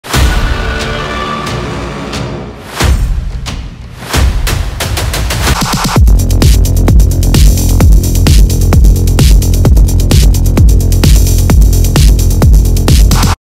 When you meet the final boss
This movie was made in the almighty Blender!
The Auto-Rig Pro addon and the Mixamo library were used for rigging/animation.

May this video feed your hungry brain...